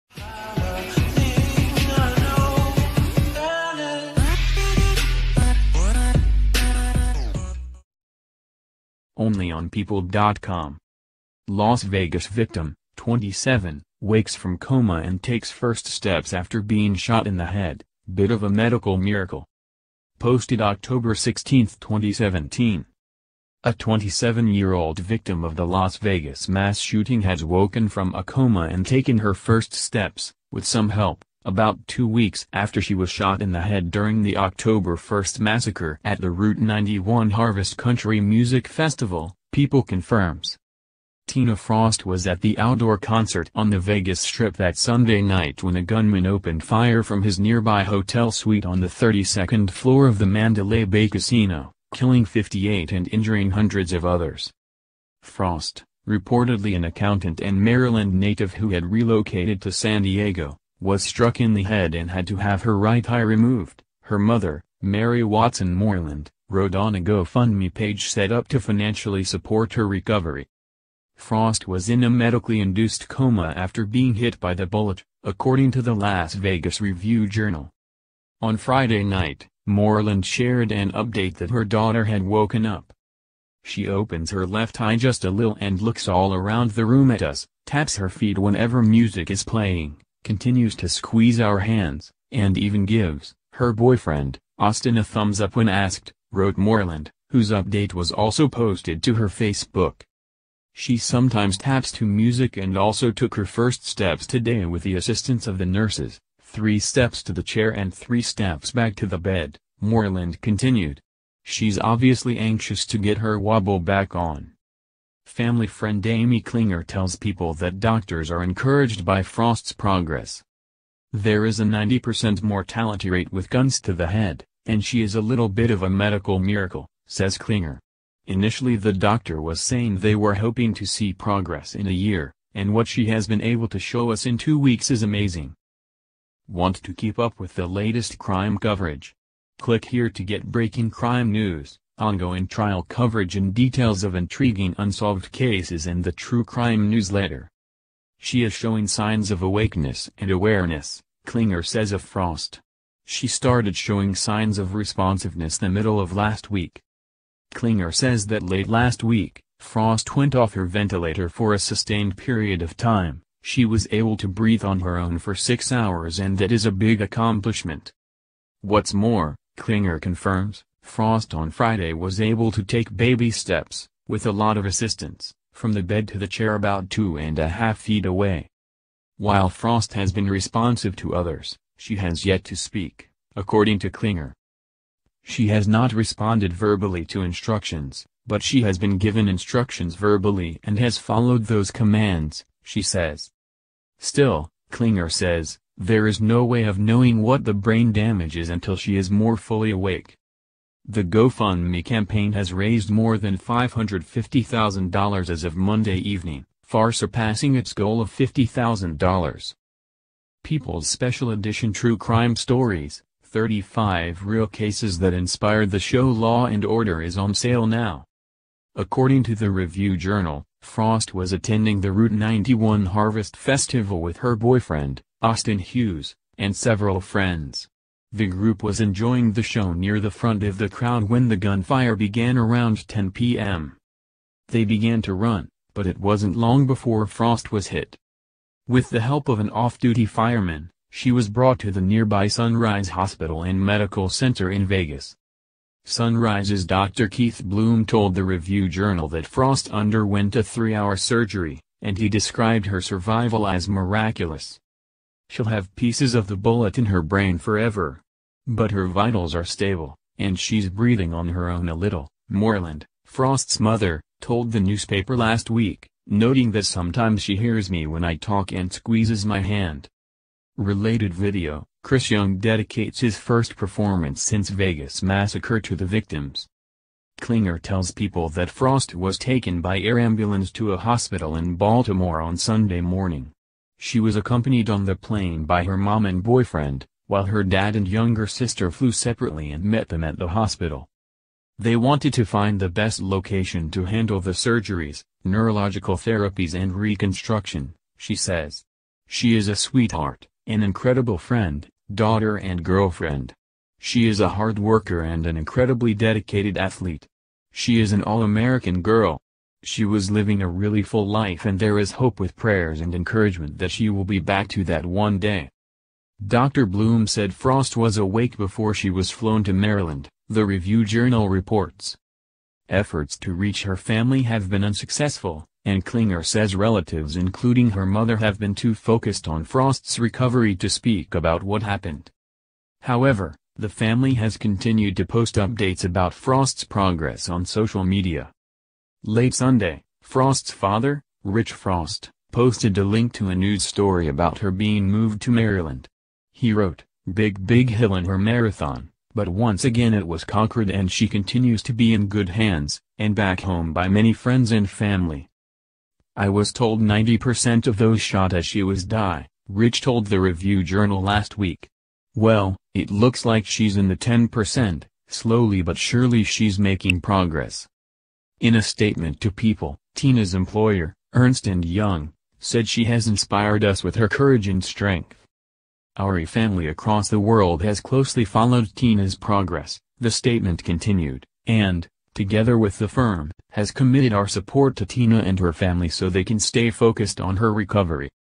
Only on People
Only on People.com

   Las Vegas Victim, 27, Wakes from Coma and Takes First Steps After Being Shot in the Head: 'Bit of a Medical Miracle'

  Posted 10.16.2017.
  A 27-year-old victim of the Las Vegas mass sho